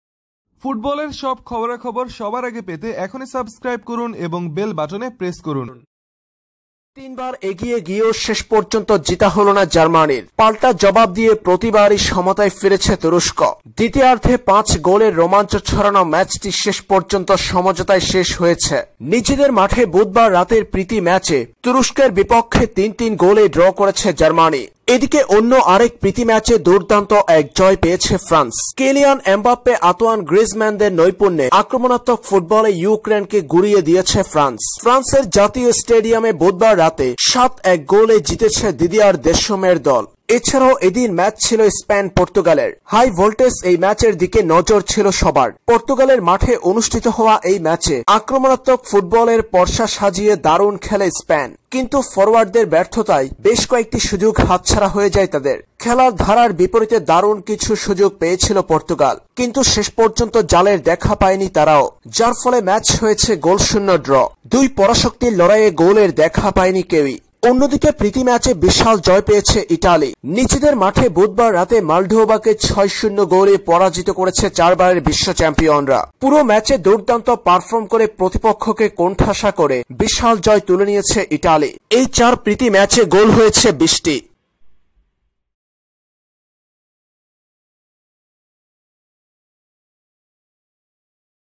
২০ গোলের ম্যাচ! লজ্জা পেল জার্মানি! ফ্রান্সের ৭ গোল! ব্যর্থ রোনালদো, ইটালির ৬ গোল | Football News
২০ গোলের ম্যাচ! লজ্জা পেল জার্মানি! ফ্রান্সের ৭ গোল! ব্যর্থ রোনালদো, ইটালির ৬ গোল | Football News
বিস্তারিত জানুন।